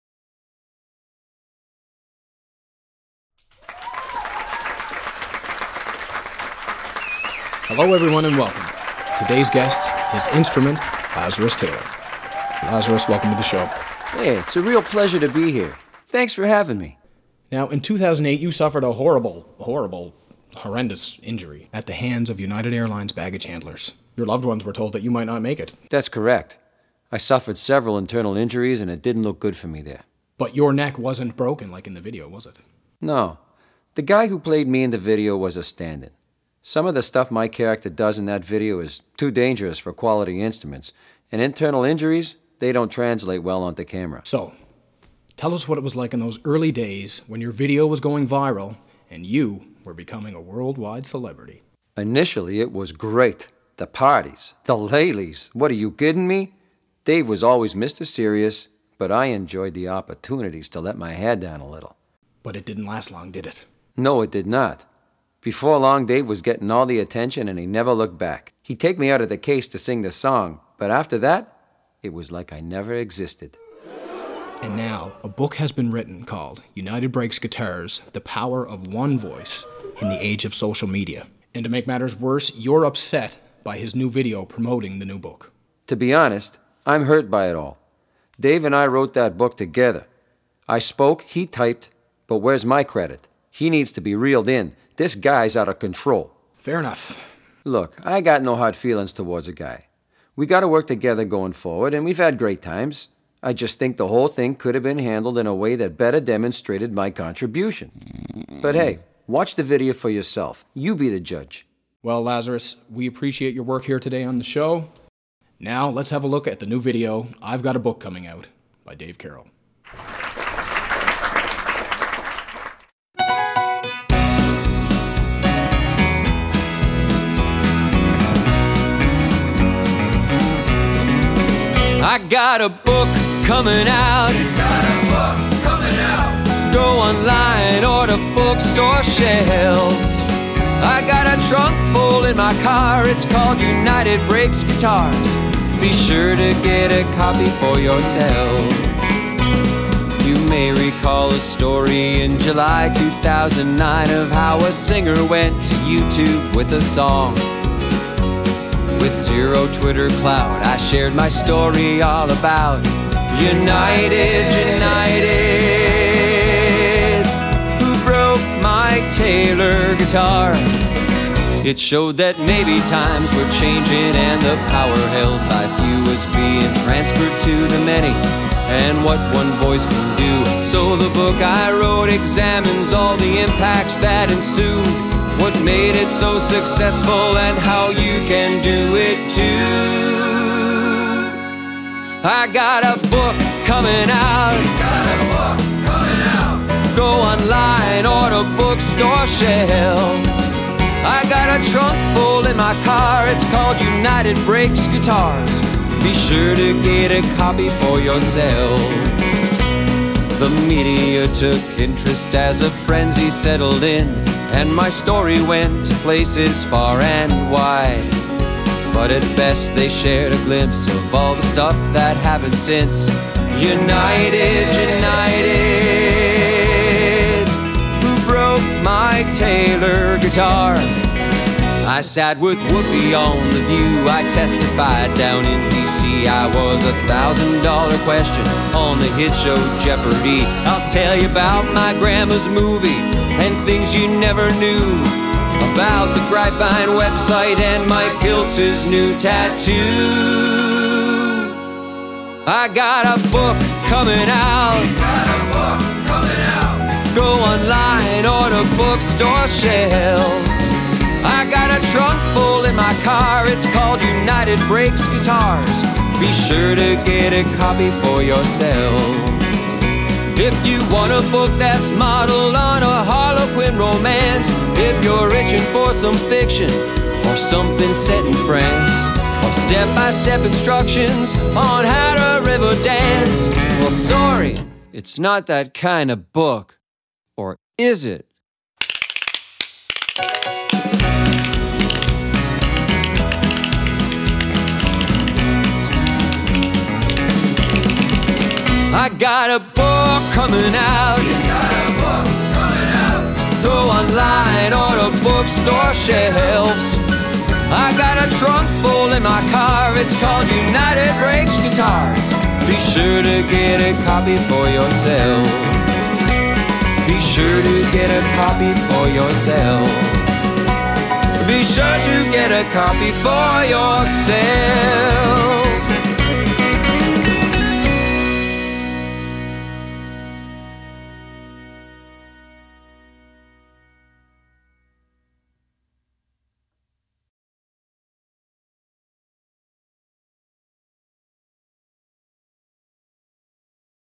I've Got a Book Comin' Out (United Breaks Guitars Book)
Purchase your Copy of "United Breaks Guitars: The Power of One Voice in the Age of Social Media" Today

Whether you are a guitarist, a baggage handler, or a boardroom executive, this book will entertain you and remind you that we are all connected, that each of us matters, and that we all have a voice worth hearing.